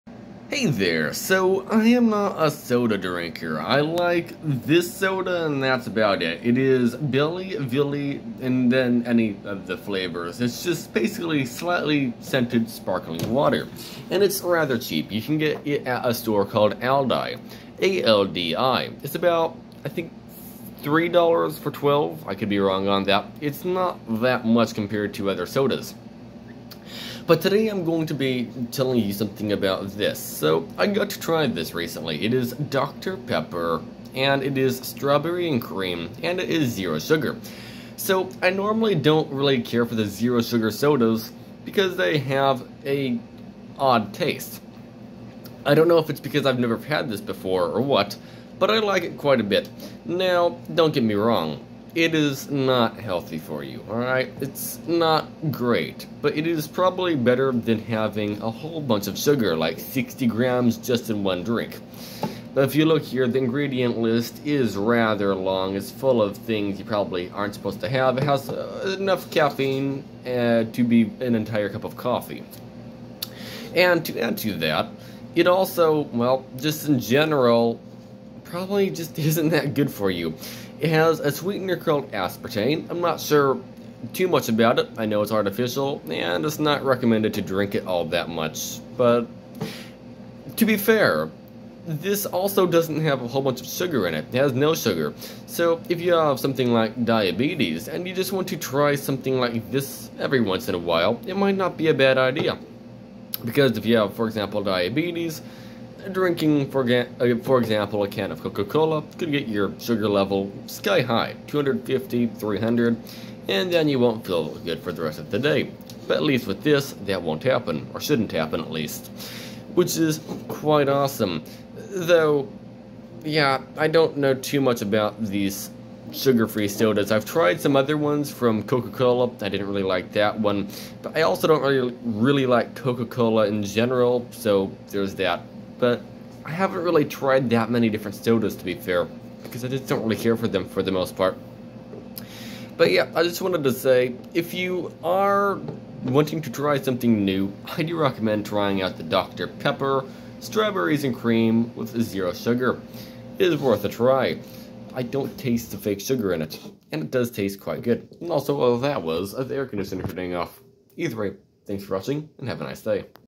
A zero sugar drink that actually tastes good?
I found out that dr. pepper zero sugar is actually a pretty good tasting drink. Yes it isn't good for you but it still does taste quite good, and it might be quite good for someone who has diabetes and just wants to taste some soda that is sweet every once in awhile.